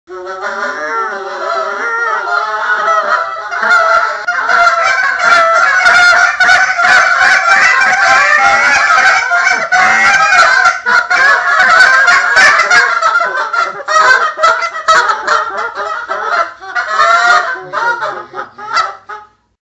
Yellow River Calls WMDs Goose Call
Here's our Promo vid for the WMDs Goose Call Debuting in Nov. Originally to debut in October, we did some last minute adjusting to make it even better. Now we have a short insert on the call and it still gets nasty! With this call...its like a bomb goes off and all the geese fall from of the sky.